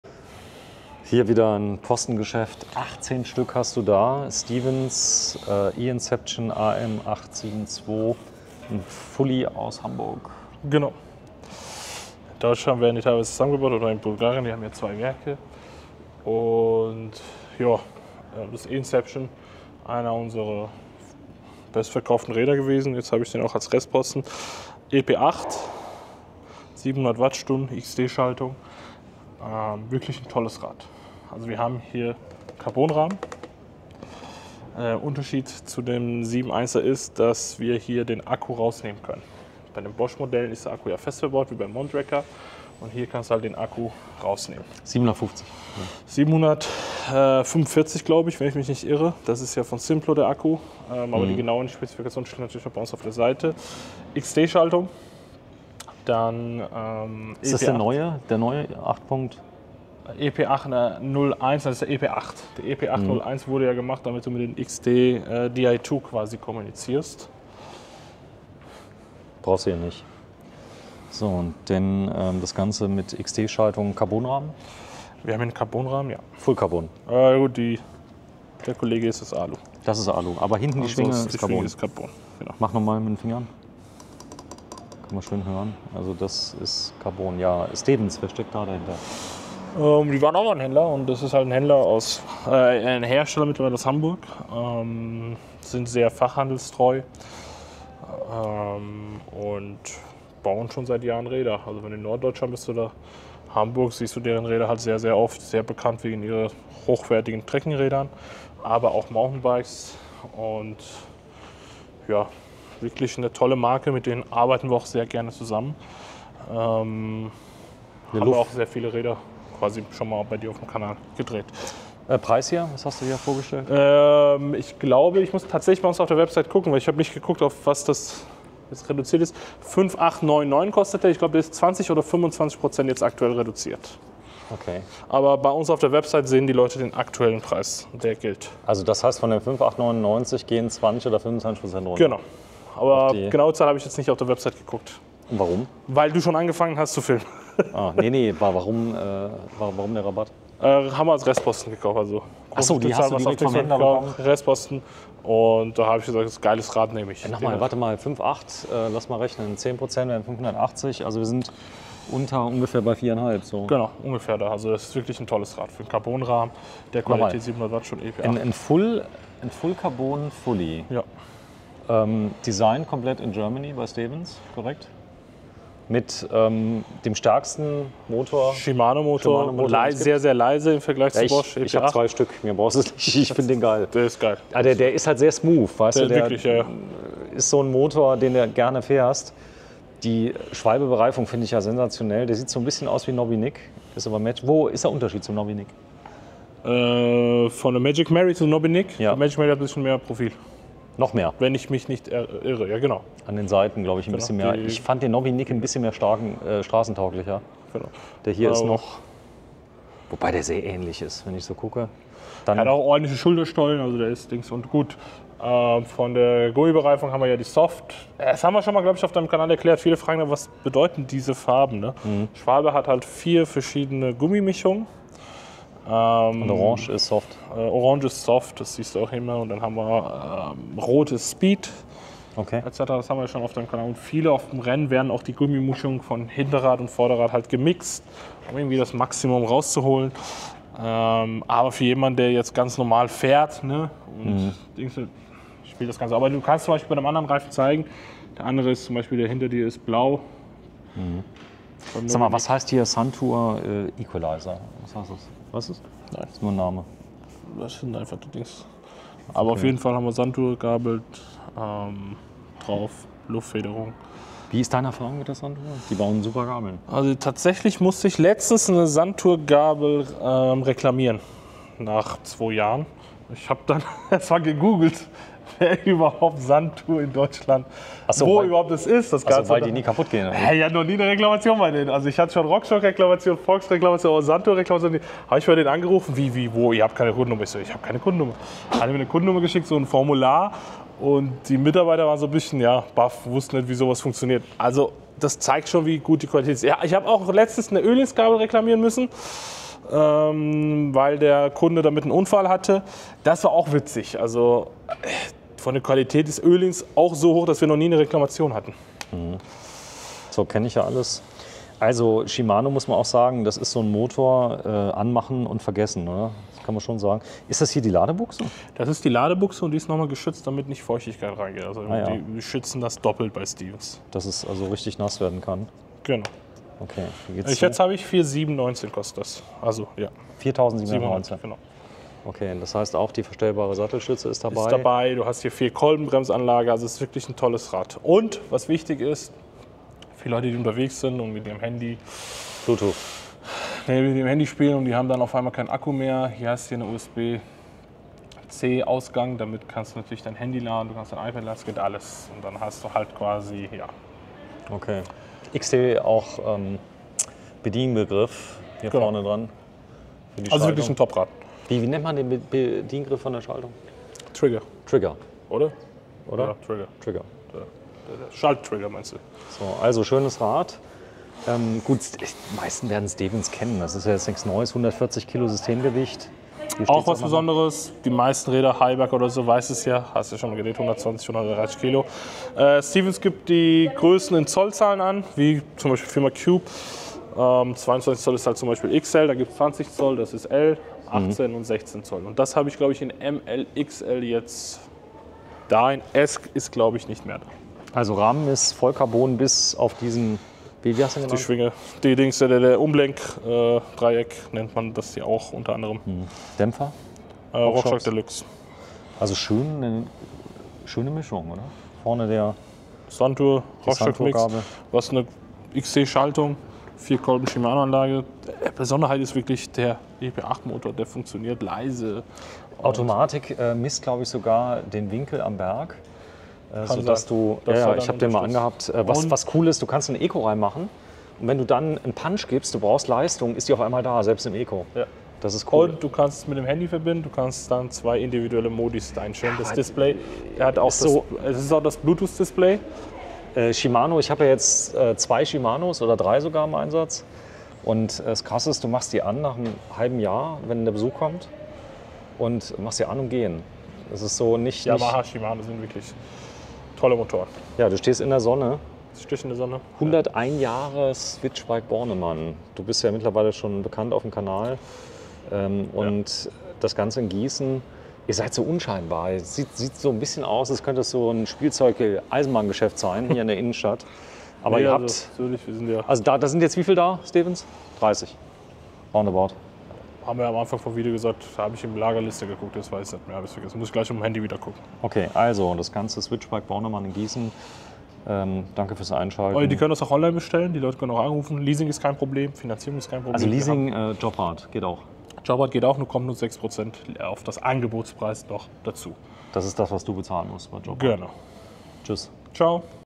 E-Bikes 2024: STEVENS E-INCEPTION AM 8.7.2 | Test und Kaufberatung
Akku:
726 Wh

Bremse:
Shimano BR-MT520 4 Kolben

Dämpfer:
RockShox Deluxe Select+

Laufradgröße:
27.5'' / 29‘‘

Rahmenmaterial:
Carbon

Schaltung:
Shimano Deore XT Shadow+ 12 Gänge

Gabel:
Sr Suntour Durolux36

Motor:
Shimano EP8 85Nm